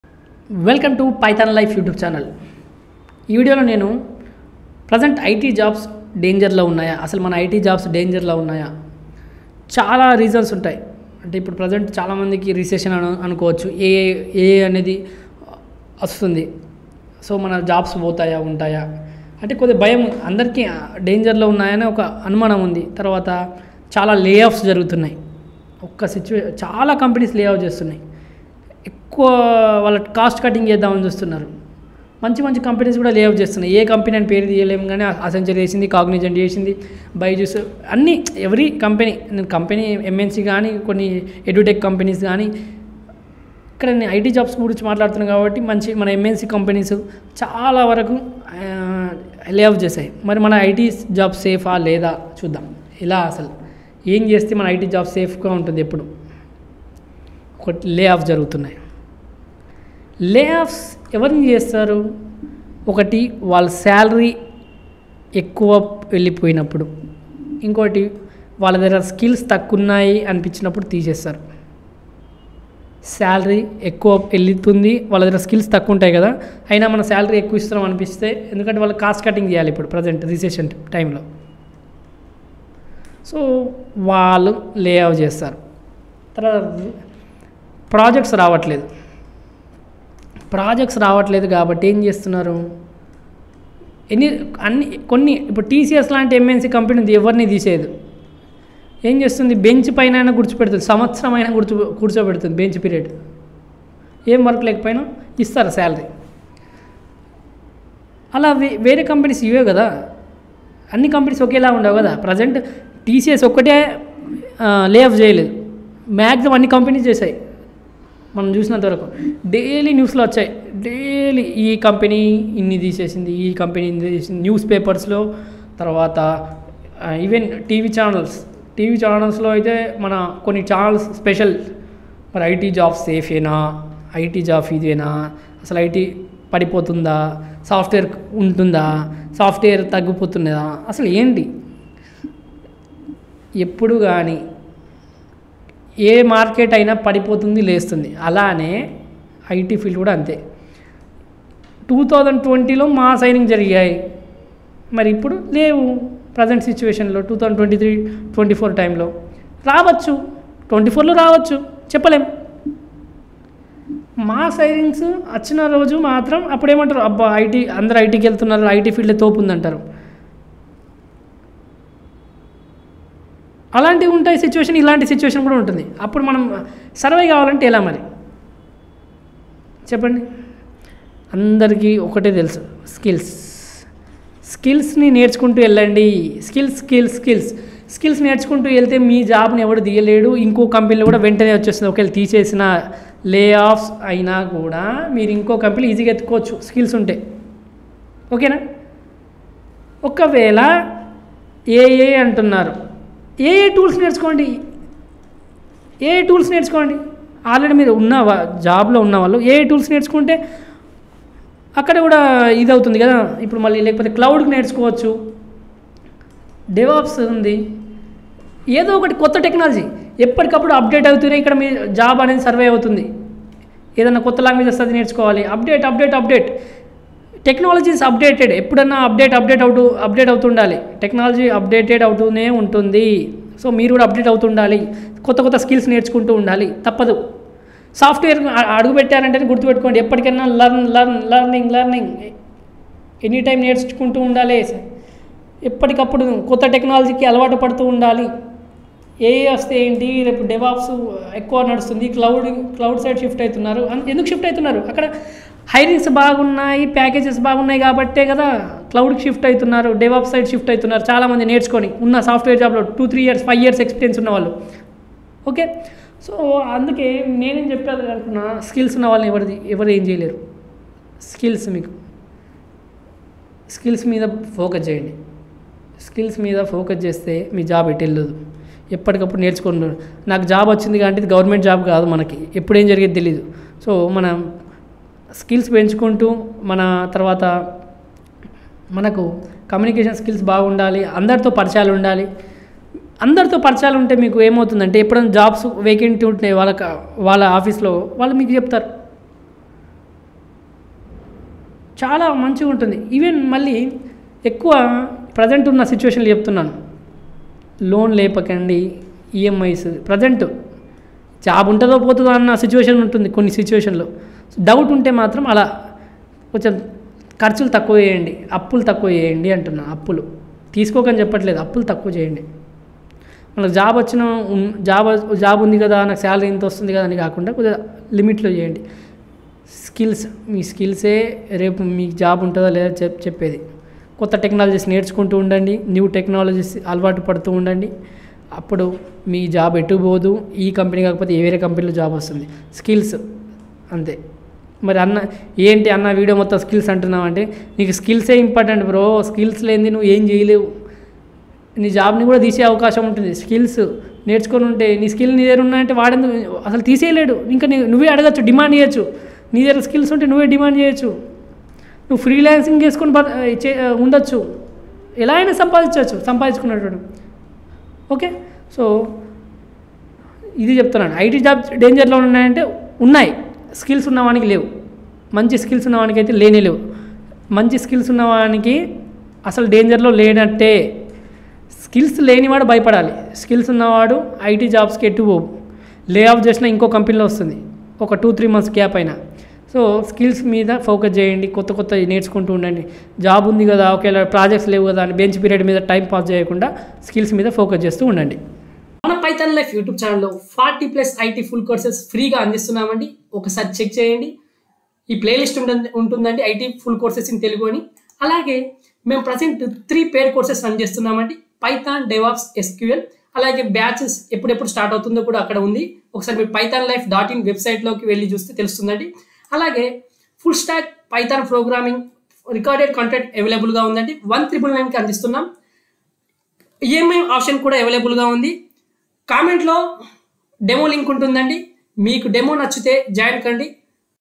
What is Our IT Job Status in Recession | Can I Lose my IT Job in Layoofs?
In this educational video, we discuss the impact of a recession on the job market, with a specific focus on the IT industry. Learn why IT jobs may be at risk during economic downturns and gain valuable insights into how to navigate these uncertain times. Don't miss out on this eye-opening discussion!